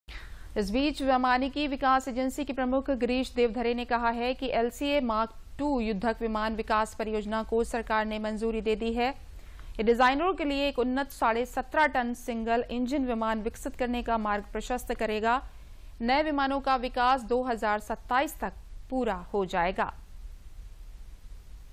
सीए माक-2 युद्धक विमान विकास परियोजना को सरकार ने मंजूरी दे दी है
इस बीच वैमानिकी विकास एजेन्सी के प्रमुख गिरीश देवधरे ने कहा है कि एलसीए माक-2 युद्धक विमान विकास परियोजना को सरकार ने मंजूरी दे दी है। यह डिजाइनरों के लिए एक उन्नत साढ़े 17 टन सिंगल इंजन विमान विकसित करने का मार्ग प्रशस्त्र करेगा। नए विमानों का विकास 2027 तक पूरा हो जाएगा